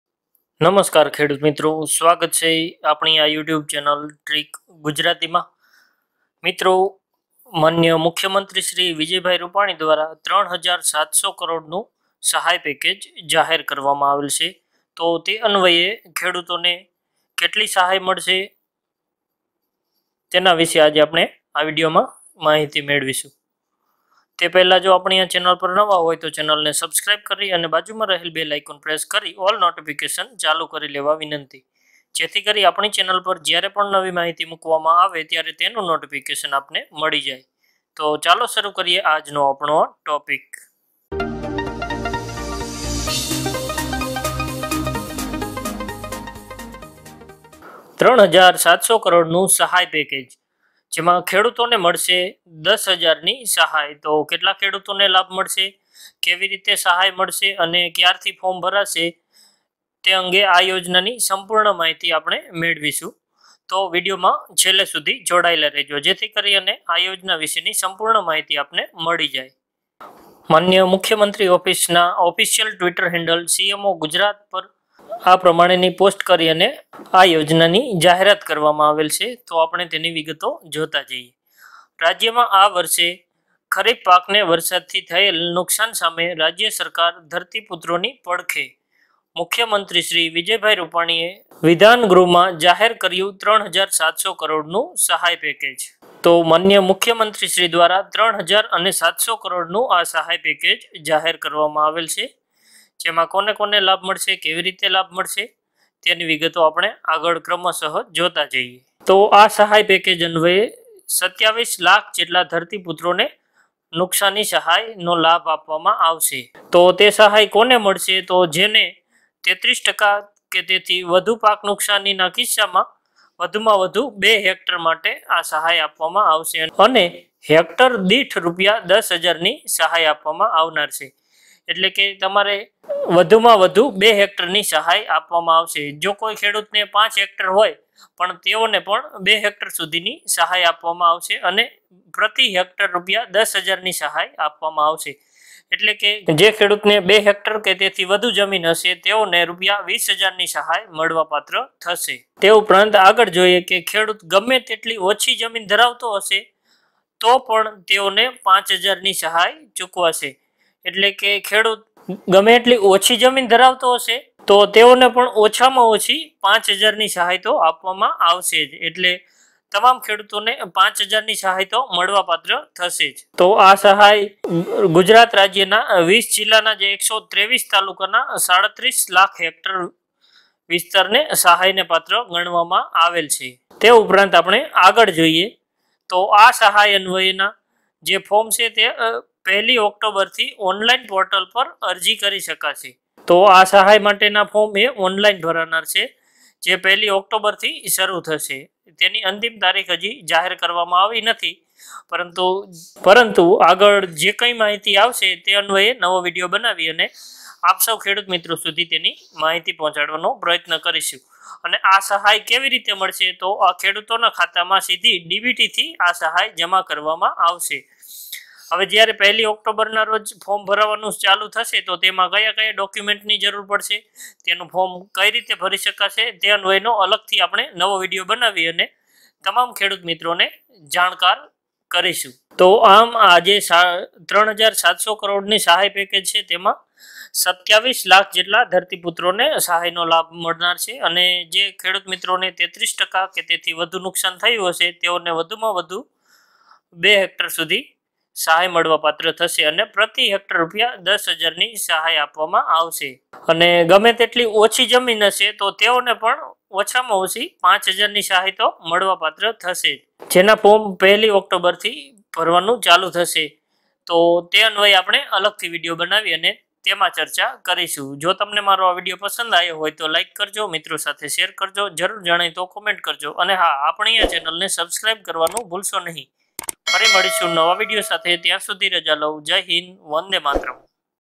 ખેડૂતોને મળશે ૧૦૦૦૦ ની સહાય || 3700 કરોડનું સહાય પેકેજ || પાક નુકશાન સહાય પેકેજ
મિત્રો આ વિડયોમાં પાક નુકશાની સહાય પેકેજ ની માહિતી આપવામાં આવી છે.